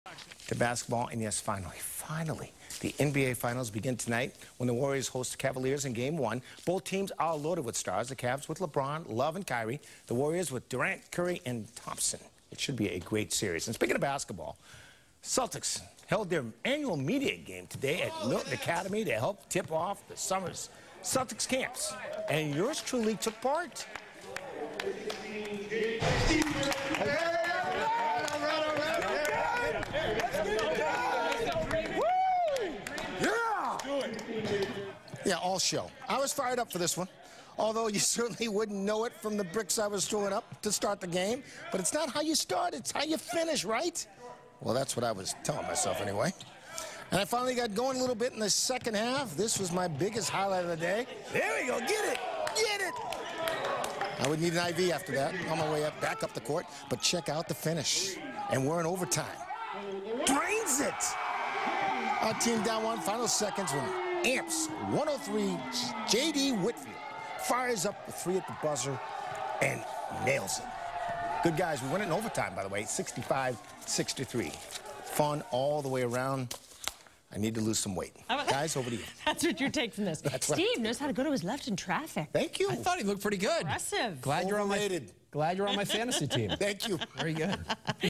Celtics Media Game Ends With Overtime Buzzer Beater
WBZ-TV's Steve Burton was among the participants in the annual game.